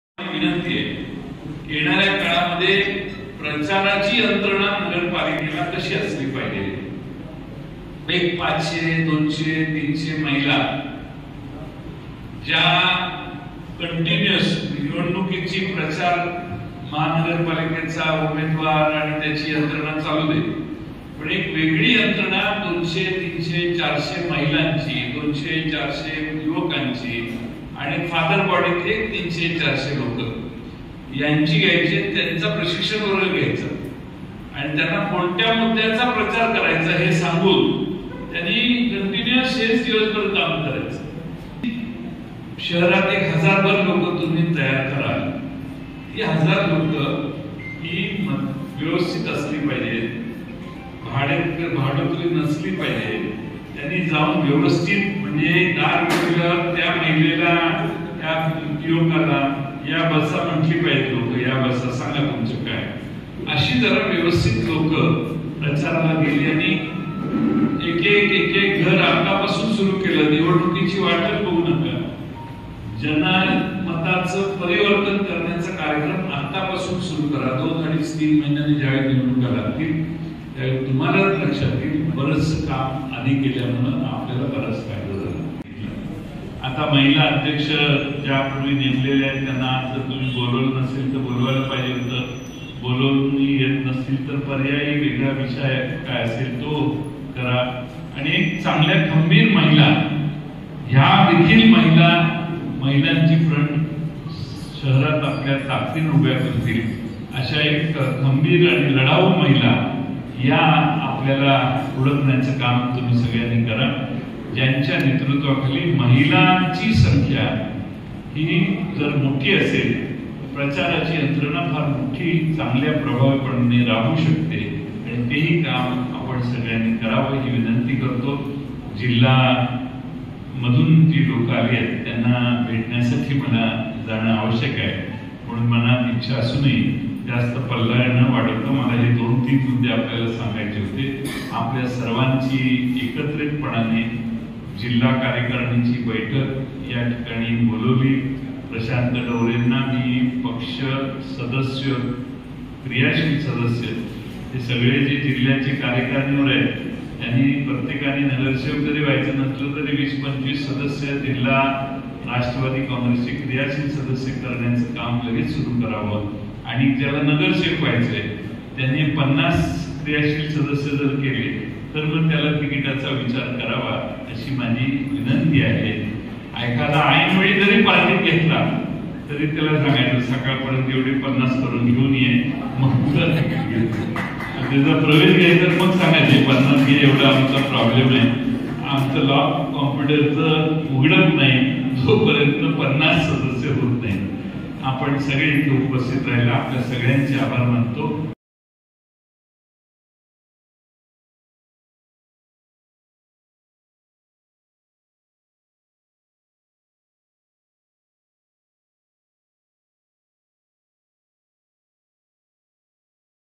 Jayant Patil : 'विकासांच्या मुद्यांवरच यश मिळवता येईल' | LetsUpp Marathi
अमरावतीमध्ये राष्ट्रवादीच्या आयोजित कार्यकर्त्यांशी संवाद बैठकीत प्रदेशाध्यक्ष जयंत पाटील यांनी कार्यकर्त्यांशी संवाद साधला. यावेळी मनपा निवडणुकीत यश मिळवायचं असेल तर काय करणं गरजेचं आहे? याबाबत माहिती दिली.